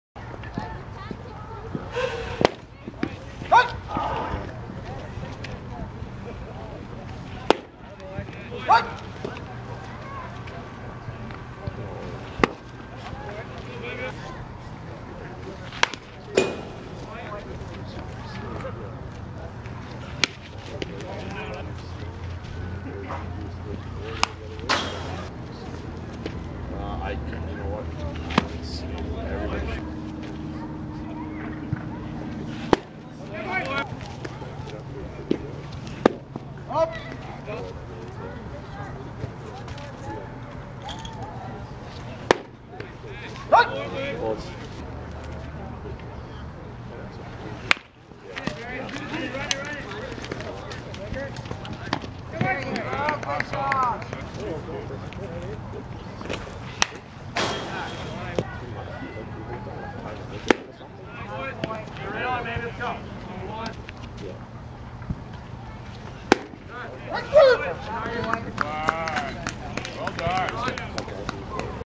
Isaac Greer (RHP, Class of 2015) Early Season Pitching
First game (exhibition) of 2015 season (early for BC baseball, a cold winter night).   Isaac pitched 1 inning, faced 3 and struck out 2 (0 Hs, 0 BB).  He maxed at 87 mph.

Exhibition Game - Whalley Chiefs v White Rock Tritons